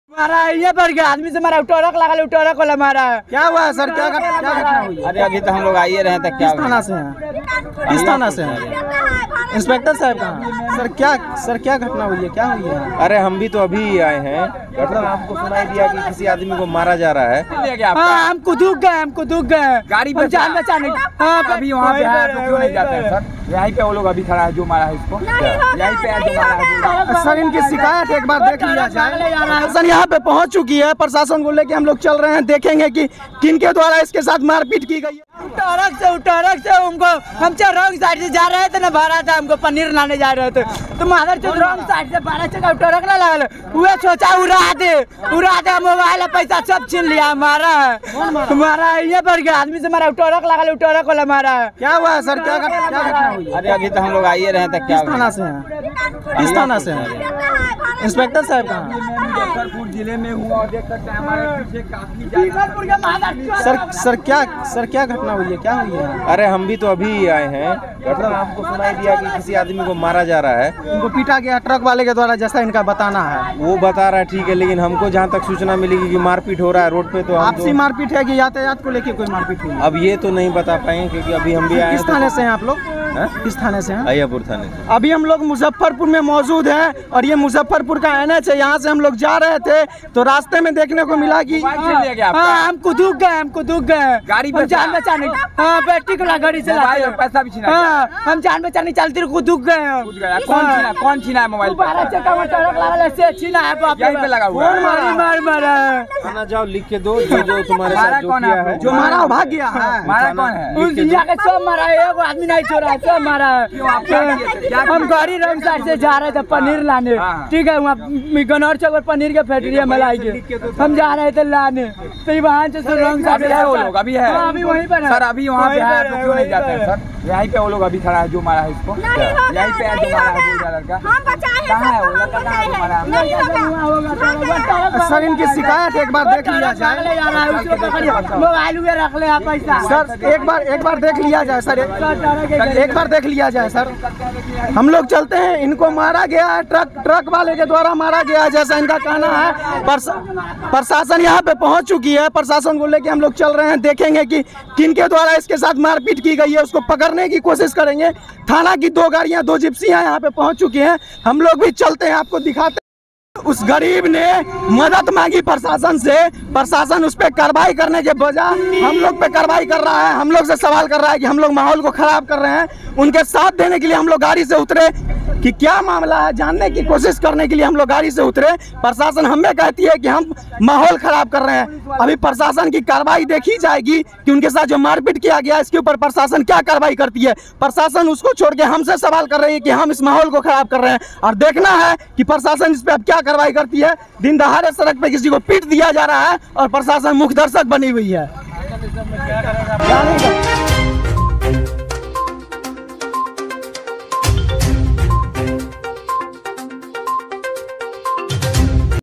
बीच सड़क पर पीट:ता रहा गरीब युवक, मुख दर्शक बनी रही बिहार पुलिस।
बीच सड़क पर पीटता रहा गरीब युवक,
मुख दर्शक बनी रही बिहार पुलिस।